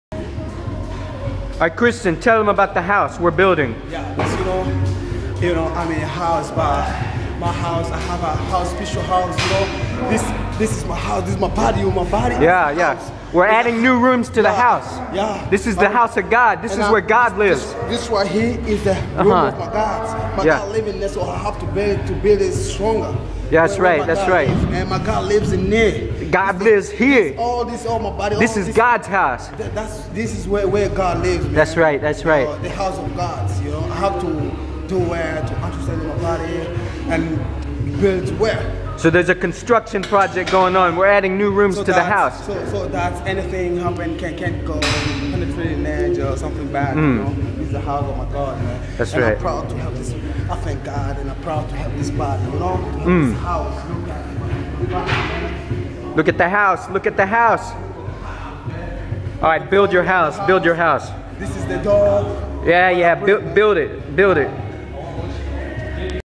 Let's build a house
In this episode of Joe at the gym, Christian builds a house.